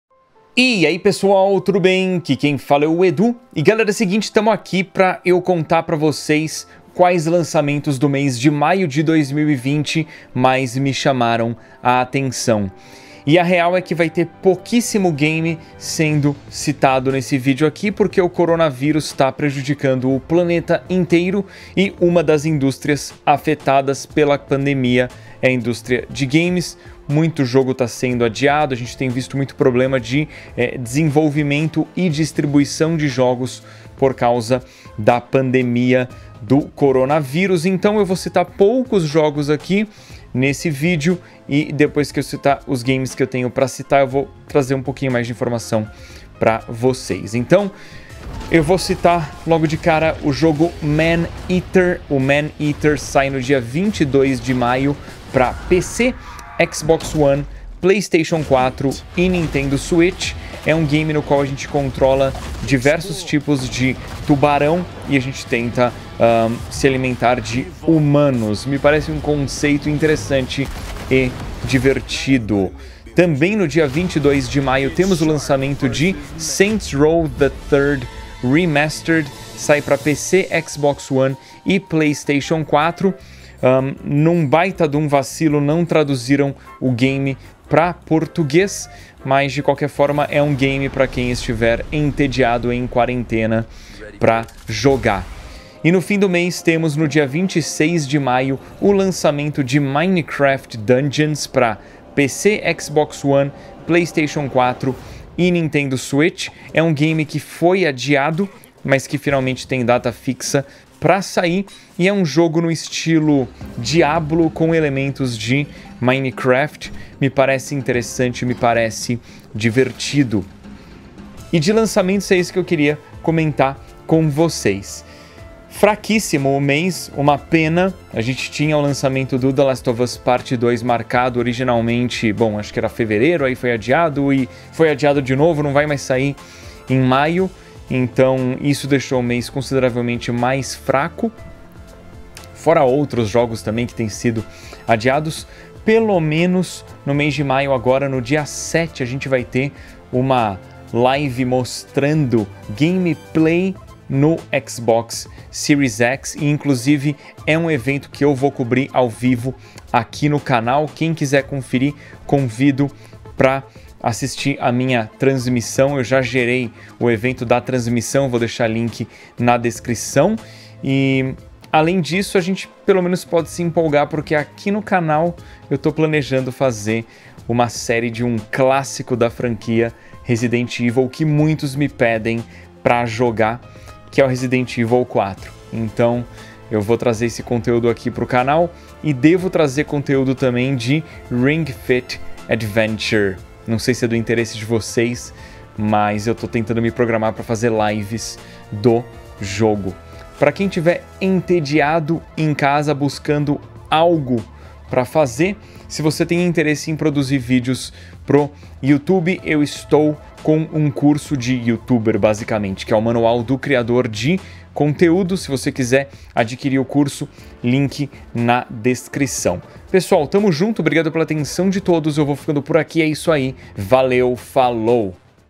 LANÇAMENTOS de MAIO de 2020 que MAIS QUERO JOGAR!
Camisetas BRKsEDU -

Vídeo falando a respeito dos lançamentos de games de Maio de 2020 que mais quero jogar. Infelizmente o mês foi bastante prejudicado pela atual pandemia, porém há bons jogos saindo para diversas plataformas!

Games mencionados:
- Maneater
- Saints Row The Third Remastered
- Minecraft Dungeons

Ouça meu Podcast: "Bora Jogar" em basicamente todas as plataformas!